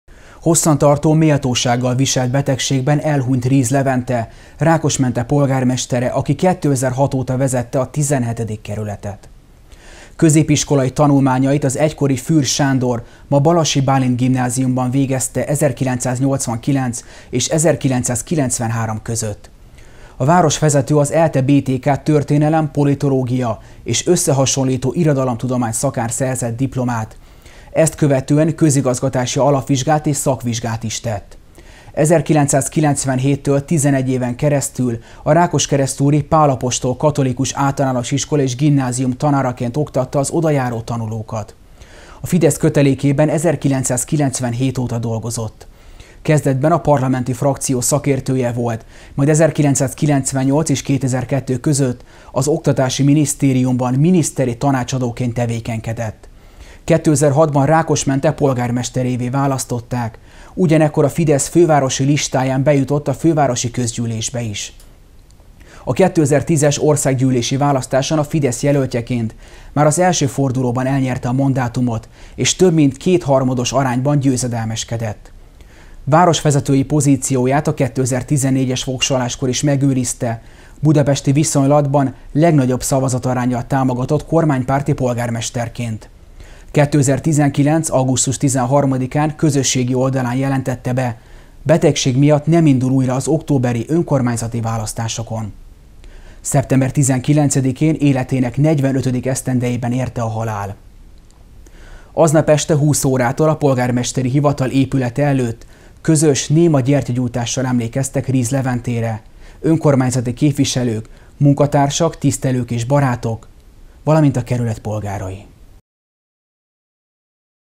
Krónika: Elhunyt Riz Levente polgármester (2019.09.20)
Hosszan tartó, méltósággal viselt betegség következtében életének 45. esztendejében elhunyt Riz Levente Rákosmente polgármestere. A városvezetőt Rákosmente Önkormányzata saját halottjának tekinti.